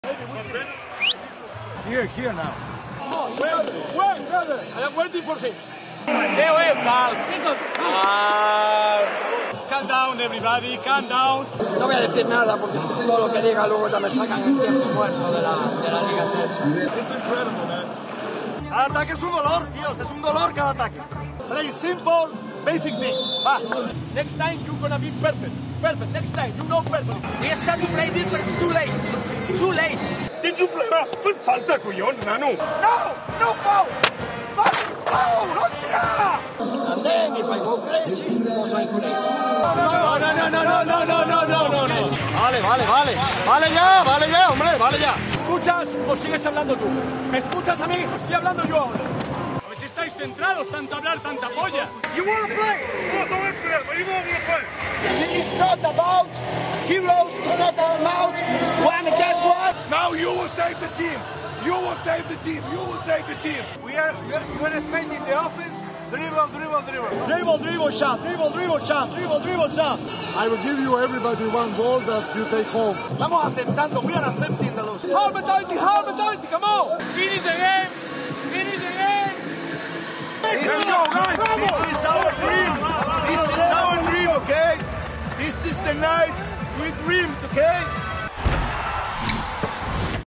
Lo MEJOR del Sonido del Banquillo 2022-23 | Liga Endesa 2022-23
Os dejamos con una selección de los mejores tiempos muertos de la temporada 2022-23. Palabras de motivación, intensidad, gestos, alguna pequeña bronca pero, sobre todo, mucha pasión por el baloncesto.